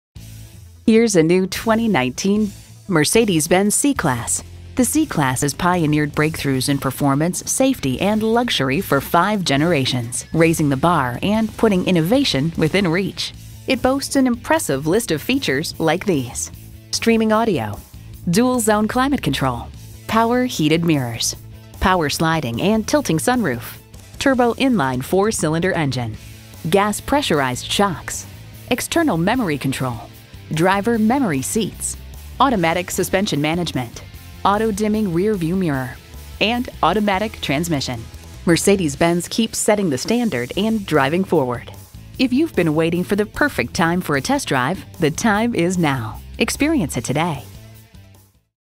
New 2019 Mercedes-Benz C-Class Silver Spring MD Washington-DC, MD
Year: 2019
Make: Mercedes-Benz
Model: C-Class
Trim: C 300
Engine: 2.0L I4 Turbocharged
Transmission: 9-Speed Automatic
Color: Black
Interior: Silk Beige
Stock #: J90463
VIN: 55SWF8EB1KU285750

Address:
3301 Briggs Chaney Rd.